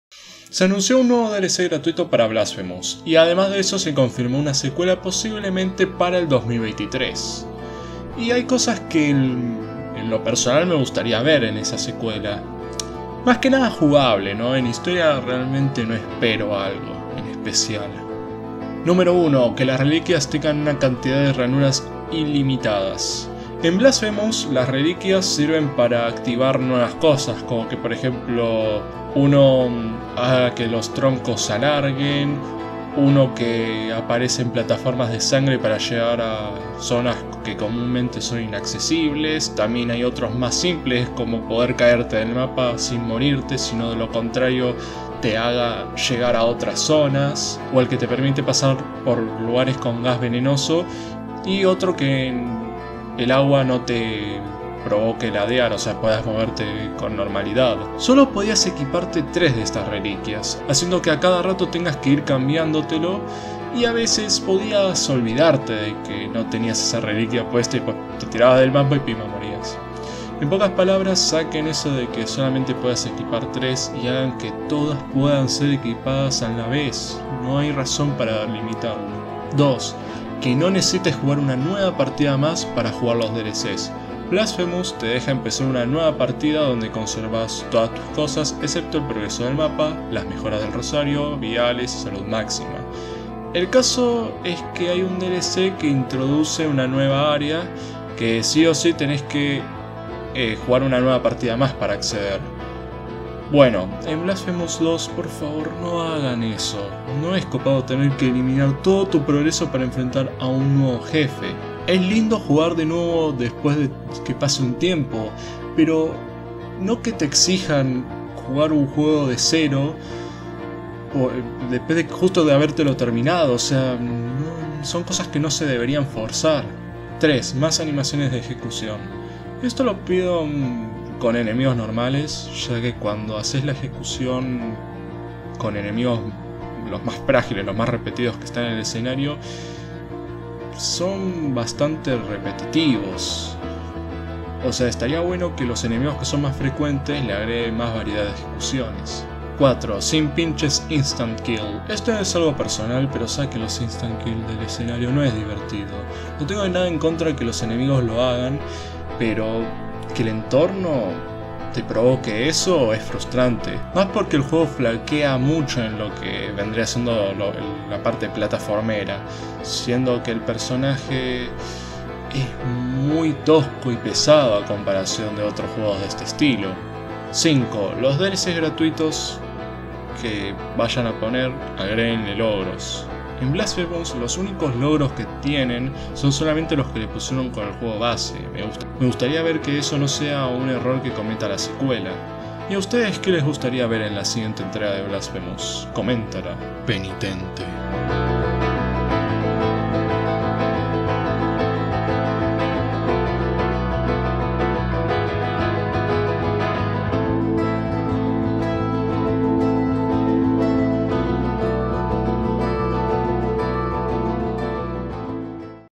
Blasphemous 5 cosas que me gustaría ver en su secuela
Edit: Creo que hay que remarcar que Blasphemous me lo di vuelta, pero aun así estás cosas no me gustaron. :p
Por otra parte, algo que podría agregar es que estaría bueno que la secuela tenga más armas. (O mejor dicho, más de una).

No voy hacer una crítica a Blasphemous ya que es un juego que simplemente me encanto, las cosas que NO me gustaron pues... Las voy a decir en el video que ahora andan reproduciendo.

Discord (Tienen que esperar que les de el rol de miembro)